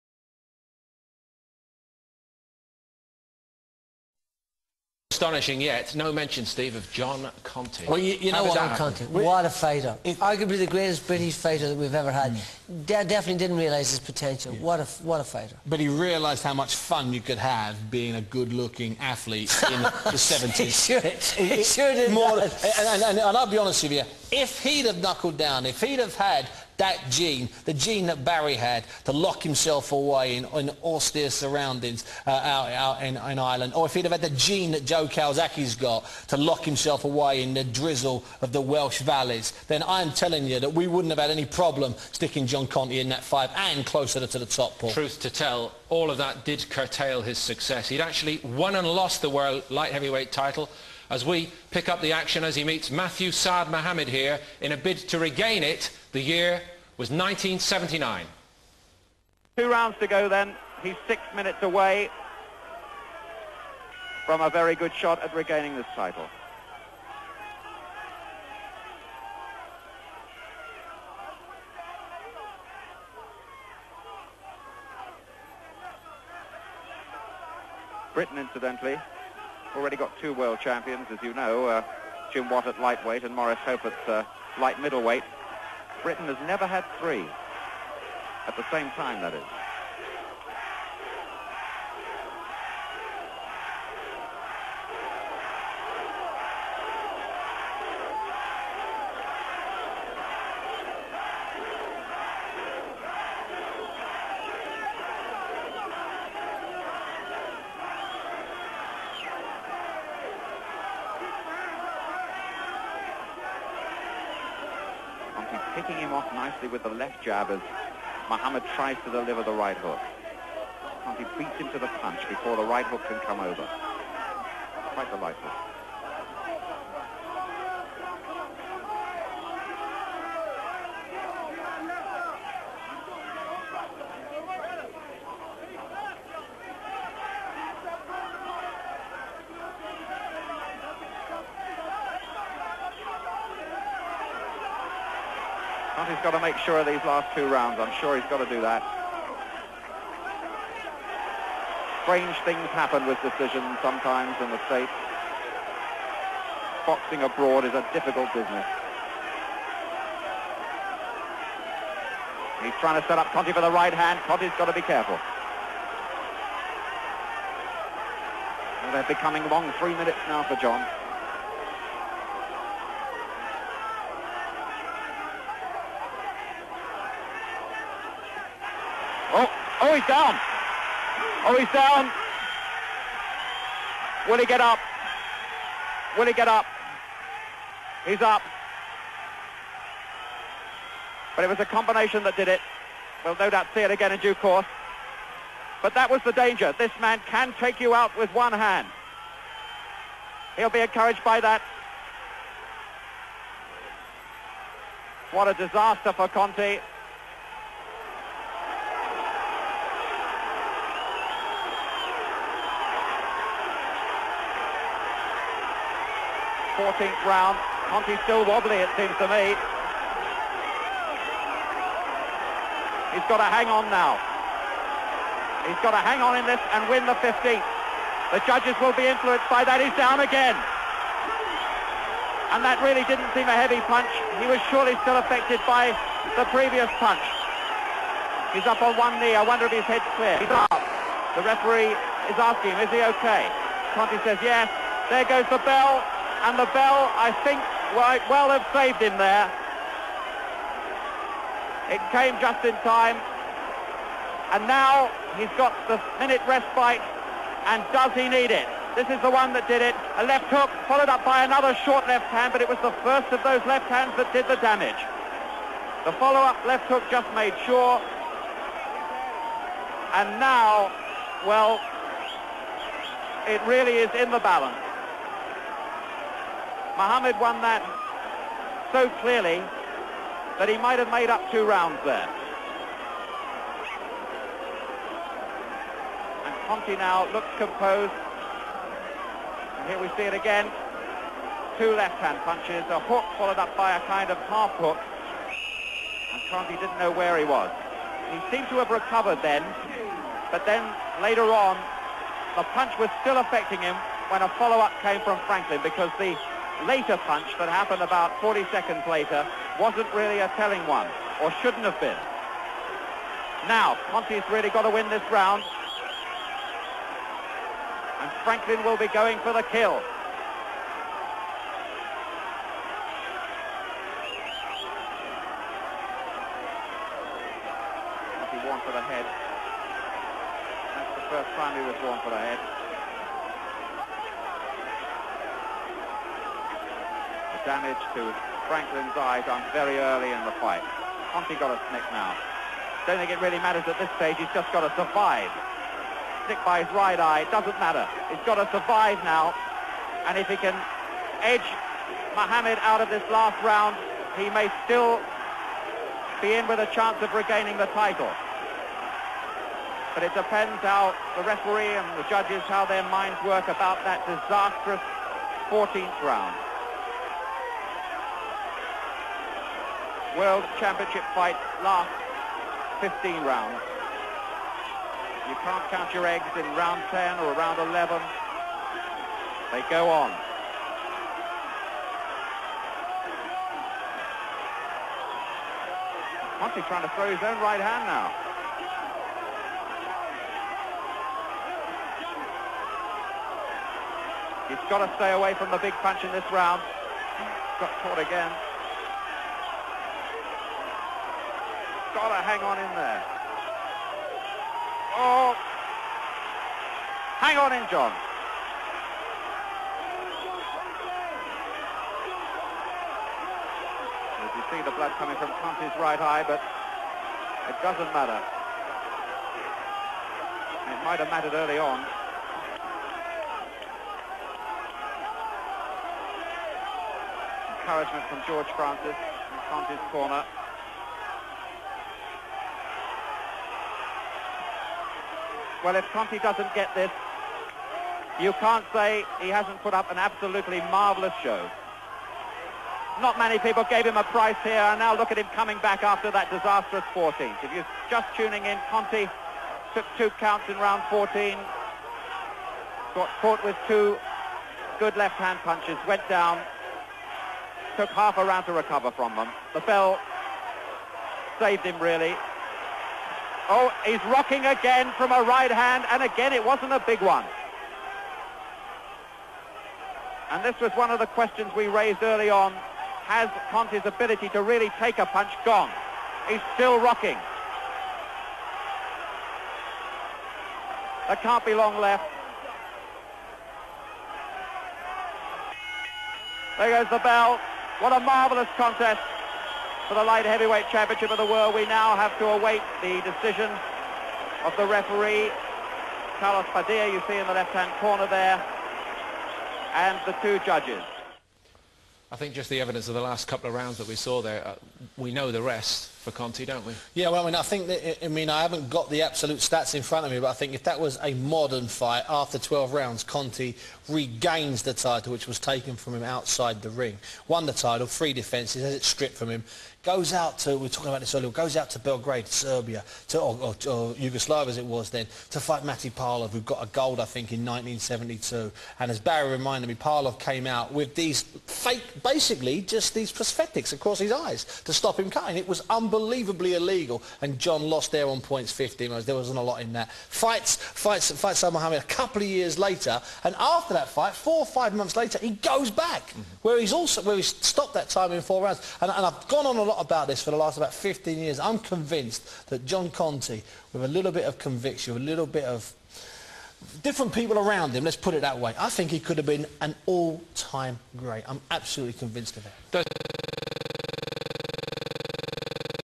John Conteh vs Matthew Saad Muhammad
Last 2 rounds of John Conteh failed attempt to win back his old World Light-Heavyweight crown from the American Matthew Saad Muhammad.